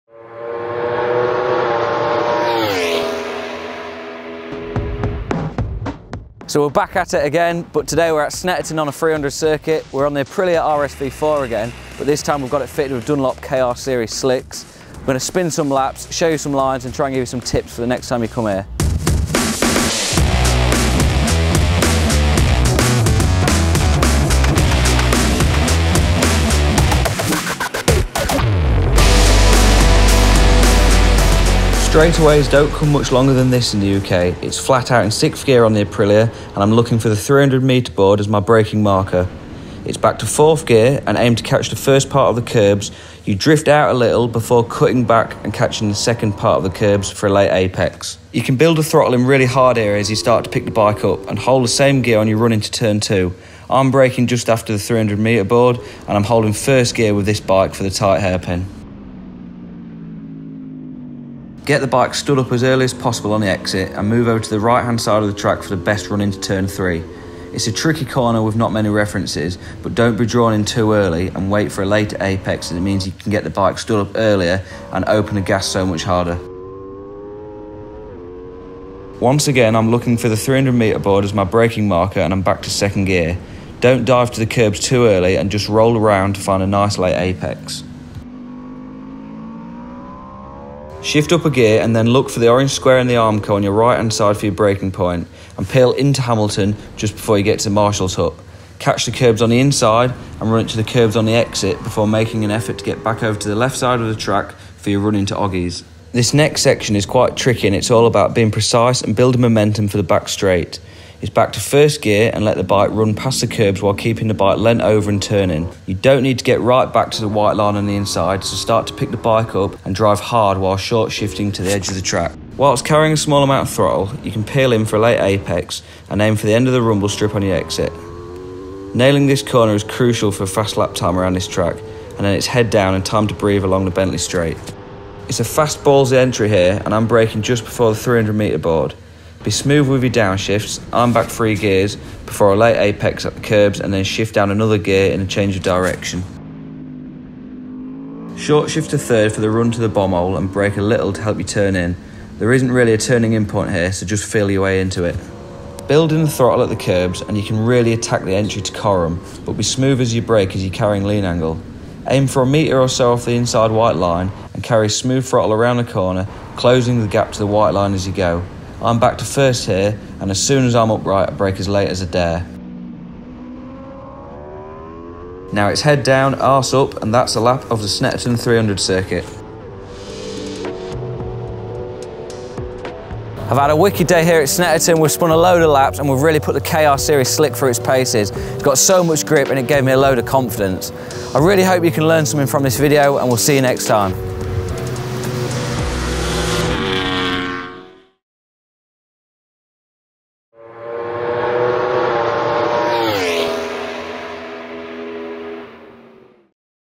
How to Tackle Snetterton Circuit | Track Guide
Tim Neave presents our best tips and tricks for how to approach Snetteron Circuit.